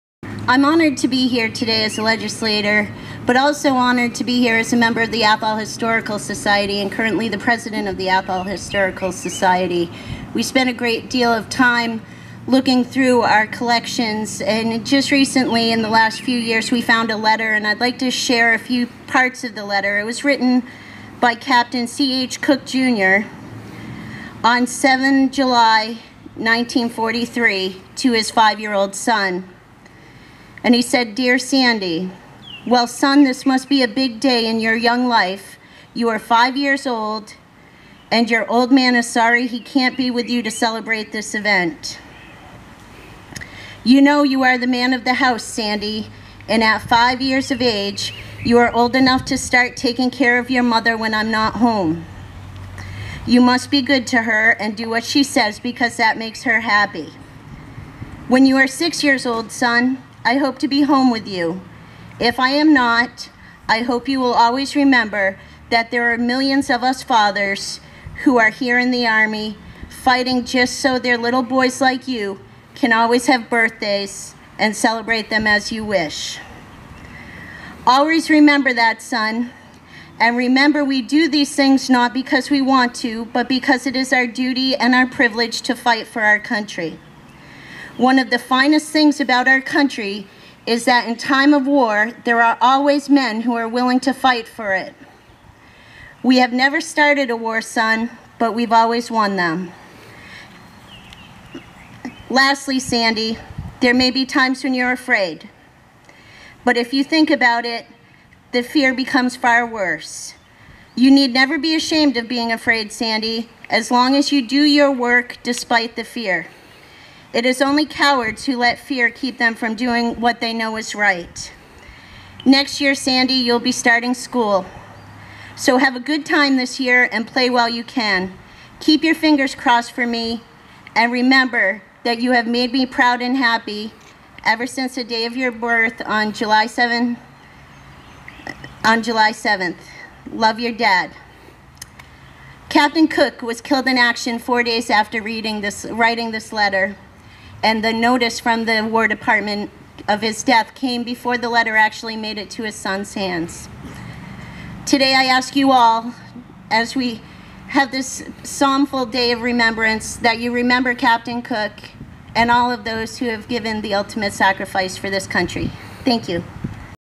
Representative Susannah Whipps QPC 26 Mar 2019
Massachusetts State Representative Susannah Whipps remarks at the 2019 Quabbin Park Cemetery Memorial Day Services held in Ware, Massachusetts. .